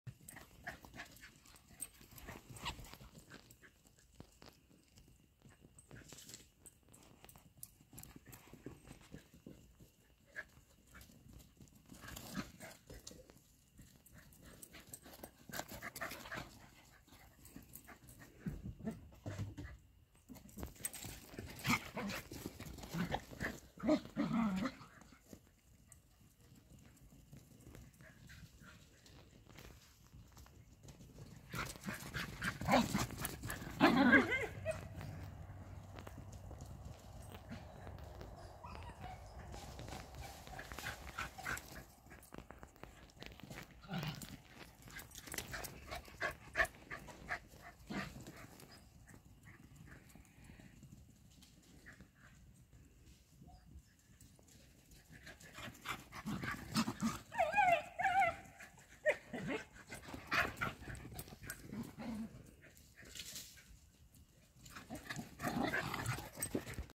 Big game of chase
Tango is the black and white Sheltie with the full white collar.